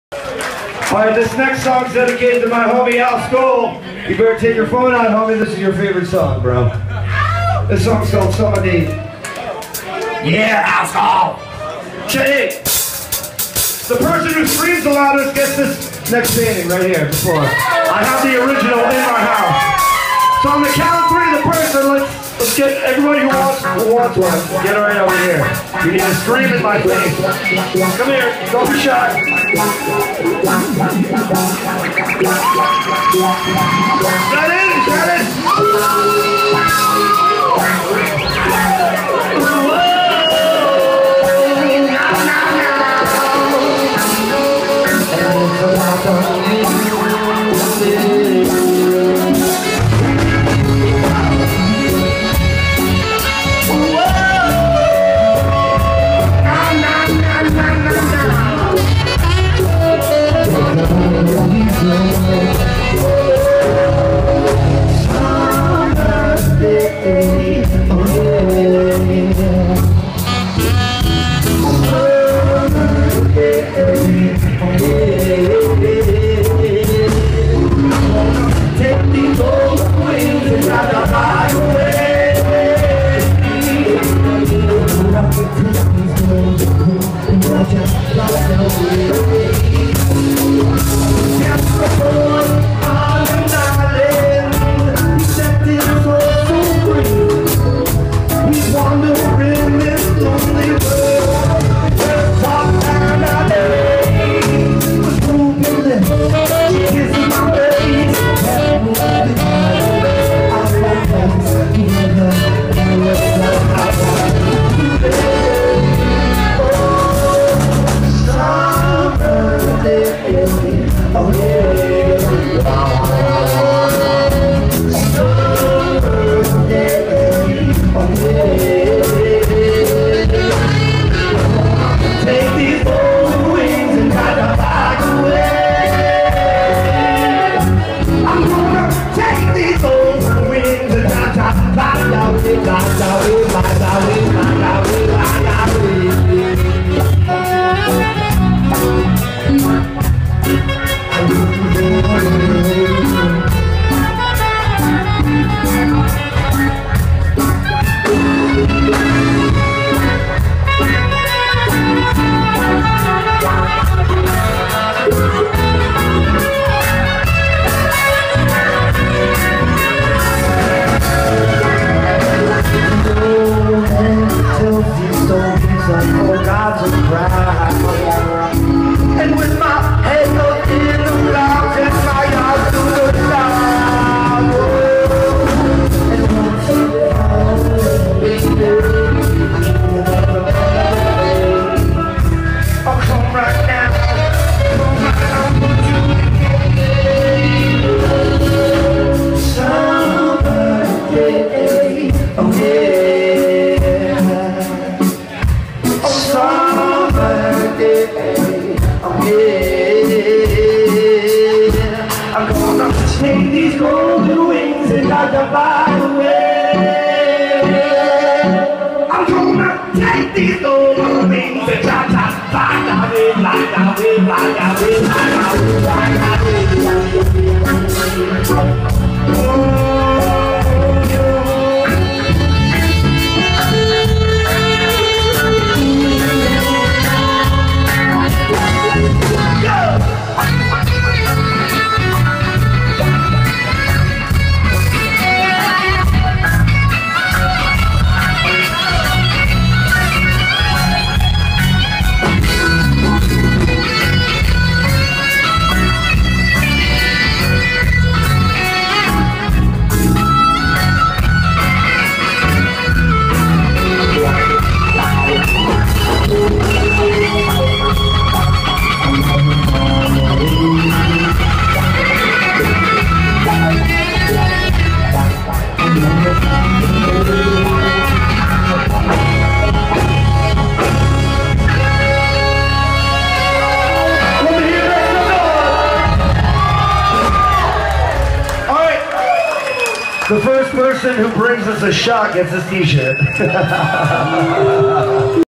Live Art Al Scholl with Sunny Rude, Summa Day @ 710 Club 7/
Live Art Al Scholl with Sunny Rude, Summa Day @ 710 Club 7/5/2014 Pay it forward my friends! Share your passion!